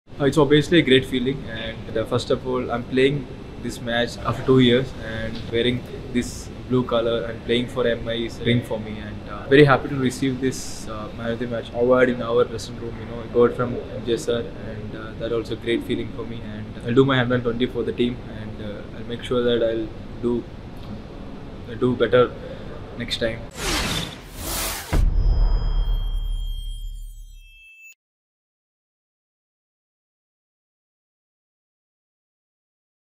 Basil Thampi - Player of the Match | Mumbai Indians
A 3️⃣-fer in a comeback match after 2 years - 💯% inspiring 💪

Basil, നിങ്ങൾ ഒരു സംഭവമാണ്! 💙